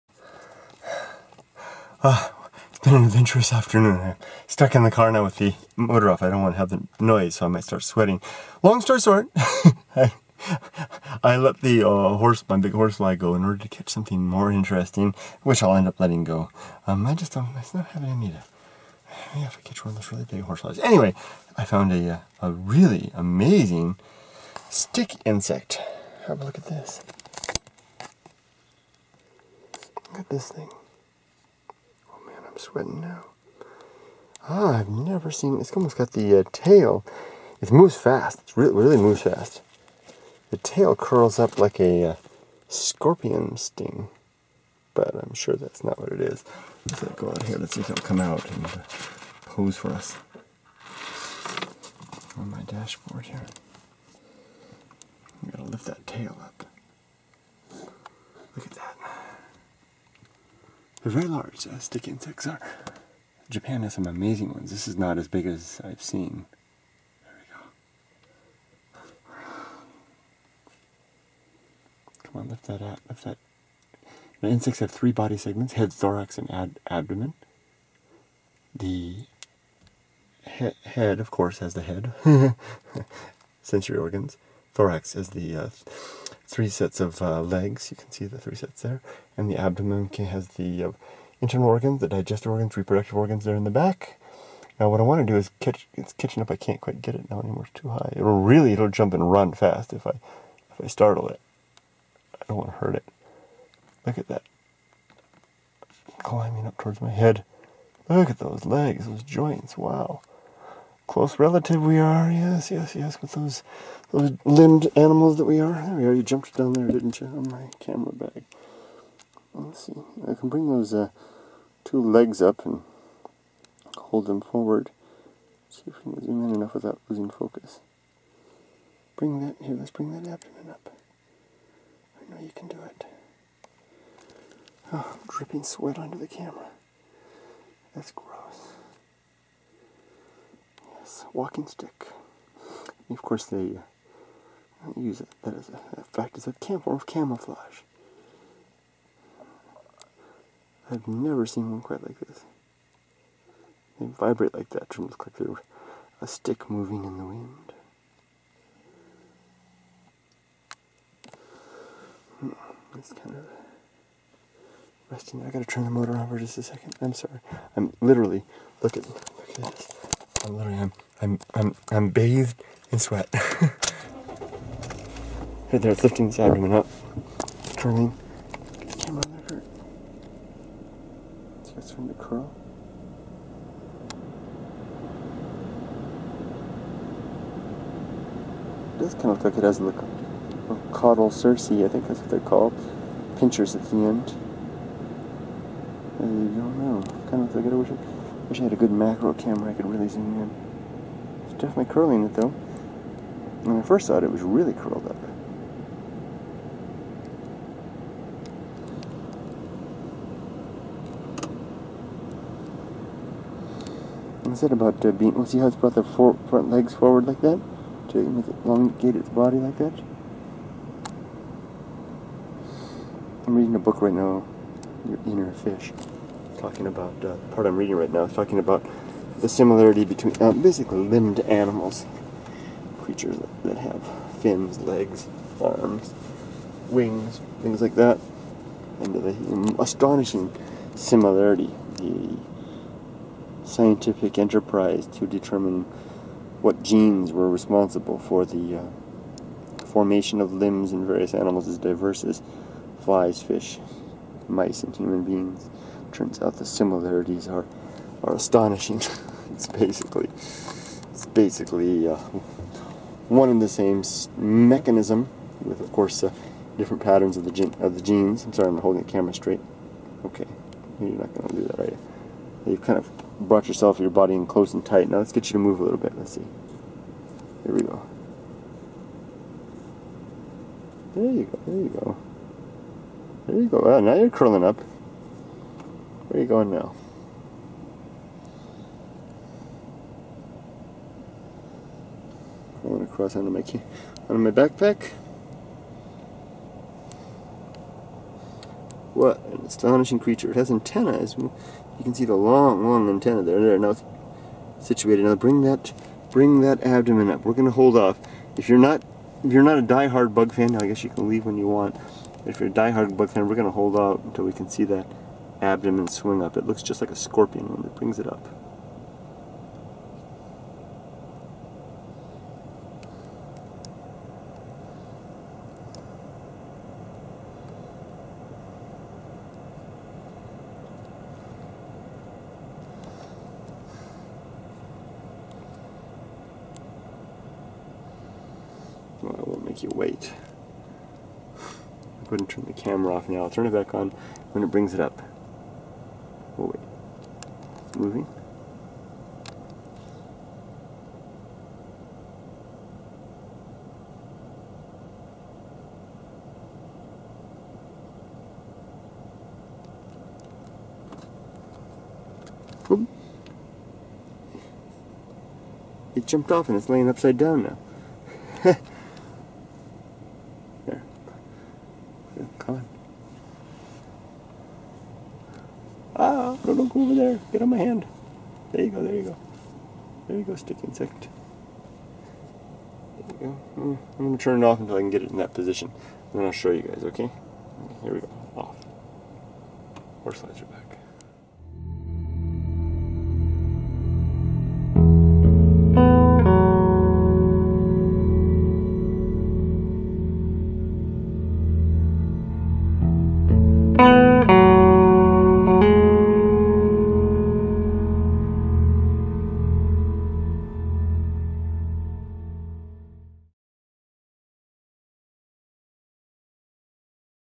🐝 Walking Stick Insect - Real Japan Monsters
🐝 Walking stick insects (Phasmatodea) are fascinating not only for their alien appearance but also for their behavior which includes rocking their bodies to appear as twigs moving in a gentle breeze. These insects are called Nanafushi ナナフシ in Japanese. The behavior of moving their bodies in a rhythmic motion may also be a clever trick to help the insect discriminate nearby objects from background clutter. Some species of stick insects are able to reproduce without males through a process called parthenogenesis. Some species of stick insect make excellent pets as they are easily reared in captivity. (KB)

Join Kurt Bell as he unravels the intricate strategies this unique insect employs to avoid predators. Witness how nature's design ingeniously blends the Nanafushi into its leafy surroundings.

➡️ Explore Kurt's insights on embracing nature's intricacies in "Going Alone"